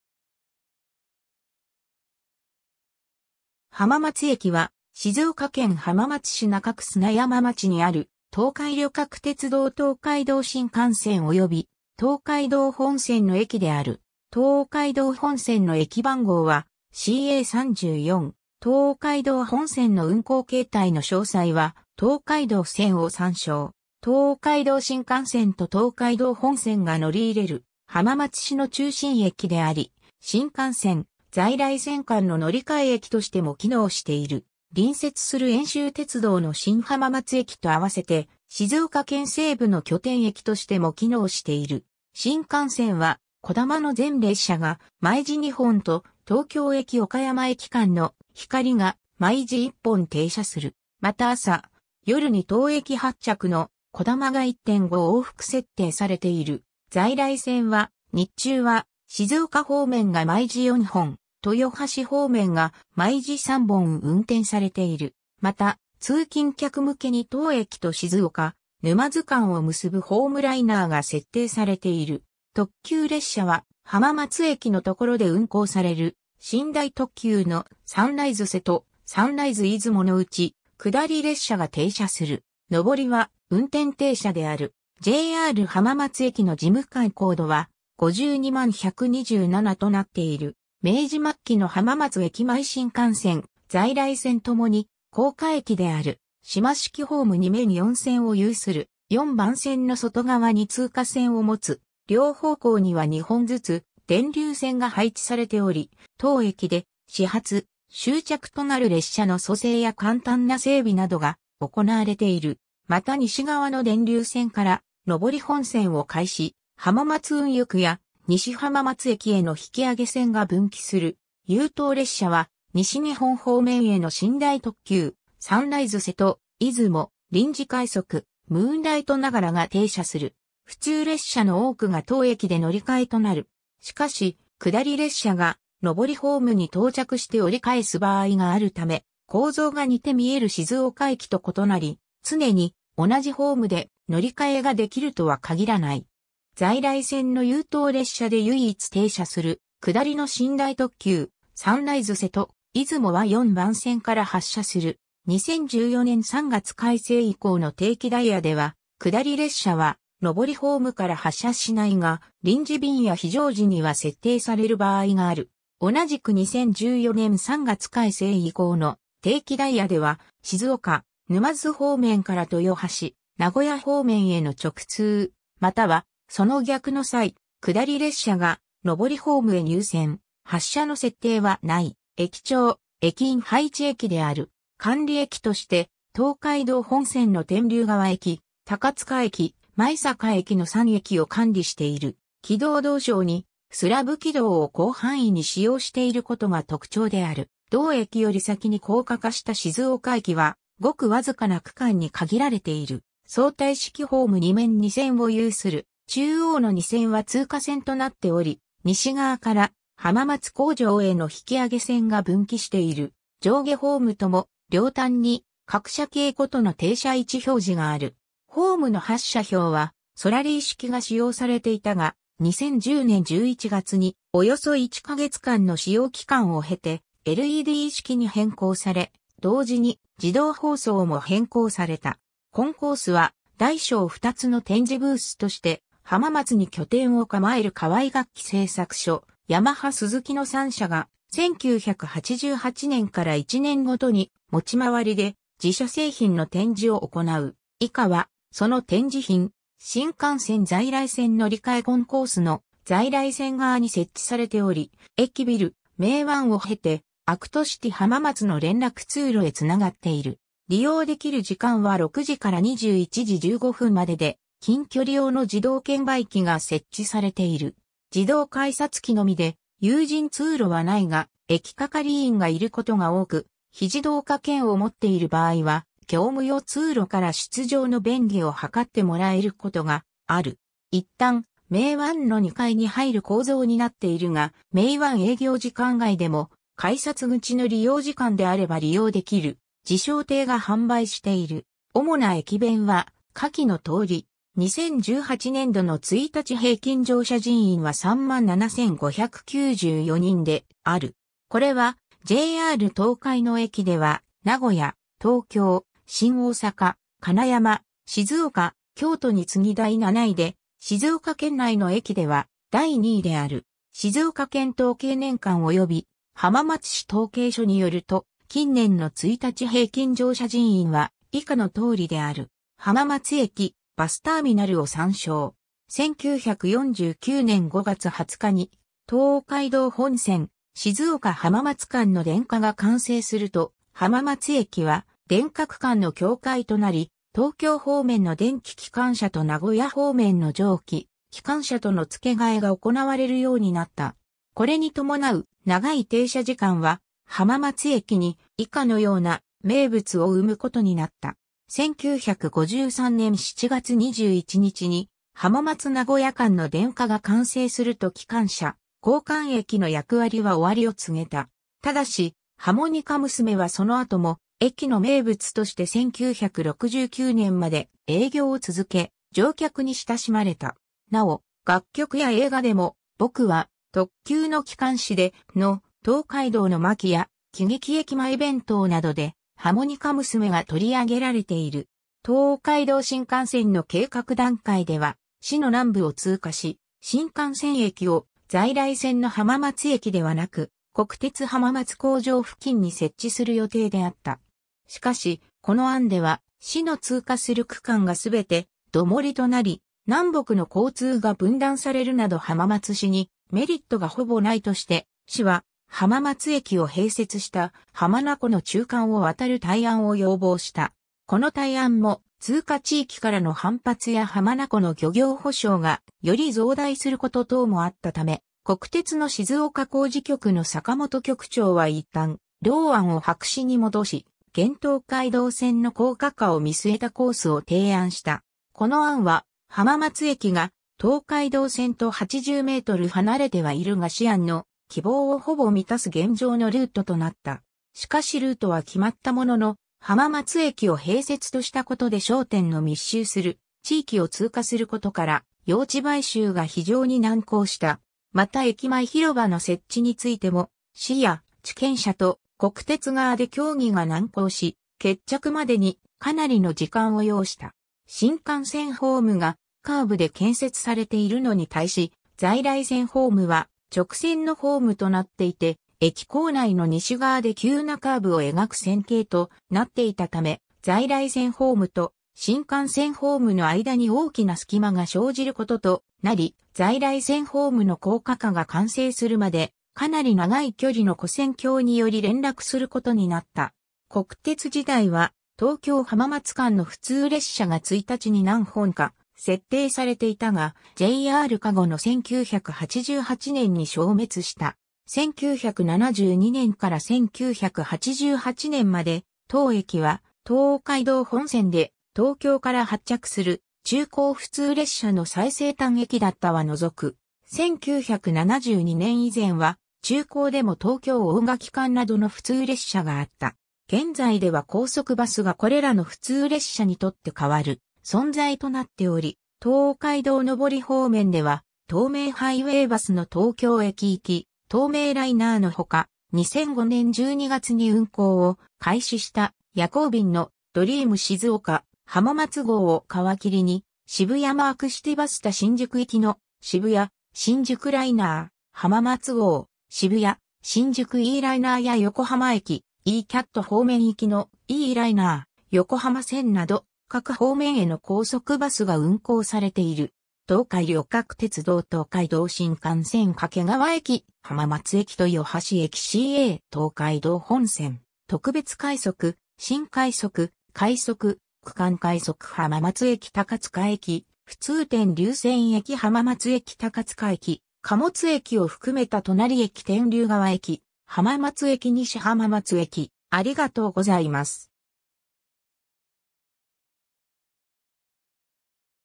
浜松駅
浜松駅（はままつえき）は、静岡県浜松市中区砂山町にある、東海旅客鉄道（JR東海）東海道新幹線および東海道本線の駅である。
東海道本線の駅番号はCA34。
 東海道本線の運行形態の詳細は「東海道線 (静岡地区)」を参照。
 東海道新幹線と東海道本線が乗り入れる浜松市の中心駅であり、新幹線・在来線間の乗換駅としても機能している。
 隣接する遠州鉄道の新浜松駅と合わせて、静岡県西部の拠点駅としても機能している。
 新幹線は、「こだま」の全列車が毎時2本と、東京駅 - 岡山駅間（一部、東京駅 - 名古屋駅・新大阪駅・広島駅間）の「ひかり」が毎時1本停車する。
また朝・夜に当駅発着の「こだま」が1.5往復（上り2本、下り1本）設定されている。
 在来線は、日中は静岡方面が毎時4本（掛川行1本,静岡以東行3本）、豊橋方面（一部、名古屋地区直通列車）が毎時3本運転されている。
また通勤客向けに当駅と静岡・沼津間を結ぶホームライナーが設定されている。
特急列車は、浜松駅のところで運行される寝台特急の「サンライズ瀬戸」「サンライズ出雲」のうち、下り列車が停車する。
上りは、運転停車である。
 JR浜松駅の事務管コードは、▲520127となっている。
 明治末期の浜松駅前 新幹線・在来線ともに高架駅である。
 島式ホーム2面4線を有する。
4番線（下り本線）の外側に通過線（下り1番線）を持つ。
両方向には2本ずつ電留線が配置されており、当駅で始発・終着となる列車の組成や簡単な整備などが行われている。
 また西側の電留線から上り本線を介し、浜松運輸区や西浜松駅への引き上げ線が分岐する。
 優等列車は、西日本方面への寝台特急「サンライズ瀬戸・出雲（下りのみ、上りは運転停車で客扱いなし）」・臨時快速「ムーンライトながら」が停車する。
 普通列車の多くが当駅で乗り換えとなる。
しかし、下り（上り）列車が上り（下り）ホームに到着して折り返す場合があるため、構造が似て見える静岡駅と異なり、常に同じホームで乗換えができるとは限らない。
 在来線の優等列車で唯一停車する、下りの寝台特急「サンライズ瀬戸・出雲」は4番線から発車する。
 2014年3月改正以降の定期ダイヤでは下り列車は上りホームから発車しないが、臨時便や非常時には設定される場合がある。
同じく2014年3月改正以降の定期ダイヤでは静岡・沼津方面から豊橋・名古屋方面への直通またはその逆の際、下り（上り）列車が上り（下り）ホームへ入線・発車の設定はない。
 駅長・駅員配置駅（直営駅）である。
管理駅として、東海道本線の天竜川駅・高塚駅・舞阪駅の3駅を管理している。
 軌道道床にスラブ軌道を広範囲に使用していることが特徴である。
同駅より先に高架化した静岡駅は極僅かな区間に限られている。
 相対式ホーム2面2線を有する。
中央の2線は通過線（本線）となっており、西側から浜松工場への引き上げ線が分岐している。
上下ホームとも、両端に各車系ごとの停車位置表示がある。
 ホームの発車標はソラリー式が使用されていたが、2010年11月におよそ1か月間の試用期間を経てLED式に変更され、同時に自動放送も変更された。
 コンコースは大小2つの展示ブースとして、浜松に拠点を構える河合楽器製作所（KAWAI）・ヤマハ・スズキの3社が、1988年から1年ごとに持ち回りで自社製品の展示を行う。
以下はその展示品。
 新幹線在来線乗り換えコンコースの在来線側に設置されており、駅ビル「メイワン」を経てアクトシティ浜松の連絡通路へつながっている。
利用できる時間は6時00分から21時15分までで、近距離用の自動券売機が設置されている。
 自動改札機のみで有人通路はないが、駅係員がいることが多く非自動化券を持っている場合は業務用通路から出場の便宜を図ってもらえることがある。
いったん「メイワン」の2階に入る構造になっているが、メイワン営業時間外でも改札口の利用時間であれば利用できる。
 自笑亭が販売している。
主な駅弁は下記の通り。
 2018年（平成30年）度の1日平均乗車人員は37,594人である。
これはJR東海の駅では名古屋、東京、新大阪、金山、静岡、京都に次ぎ第7位で、静岡県内の駅では第2位である。
 「静岡県統計年鑑」及び「浜松市統計書」によると、近年の1日平均乗車人員は以下の通りである。
 浜松駅バスター...